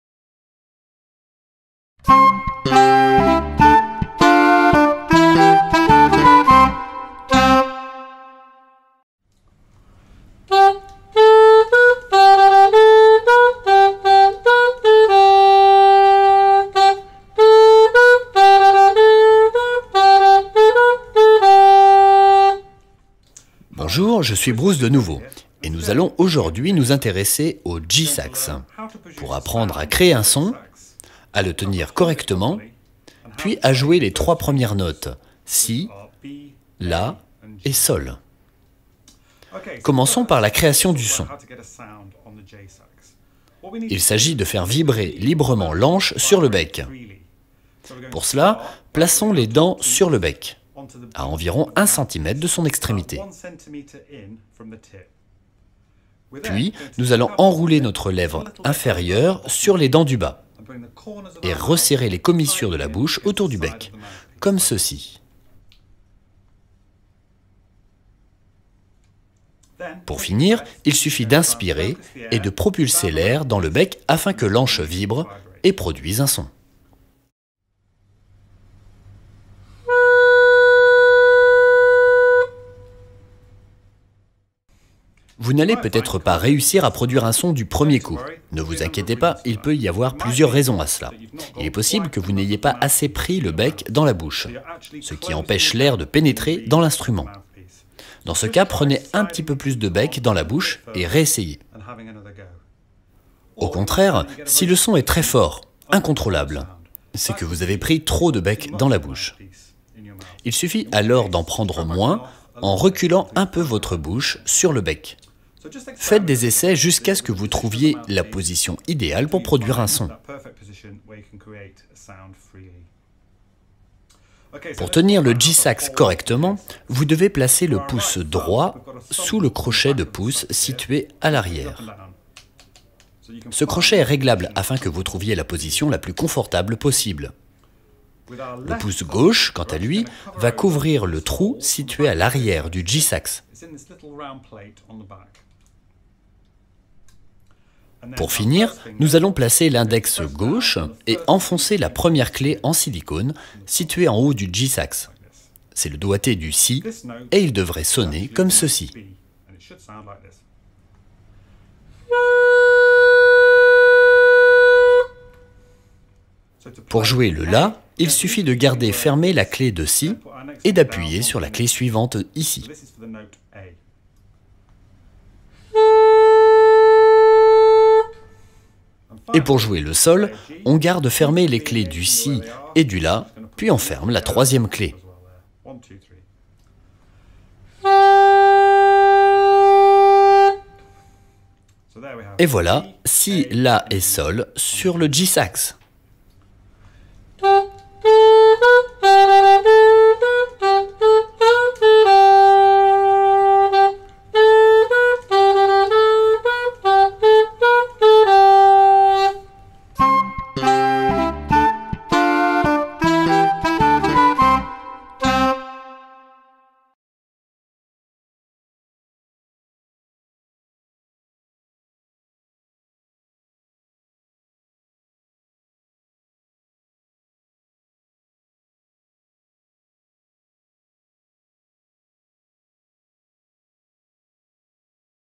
NUVO - Jouer de votre jSAX (vidéo de La Boite Noire)
Comment bien se placer et tenir l'instrument jSAX NUVO.

Le jSAX est un instrument idéal pour les débutants souhaitant s'initier au saxophone, notamment auprès des plus jeunes.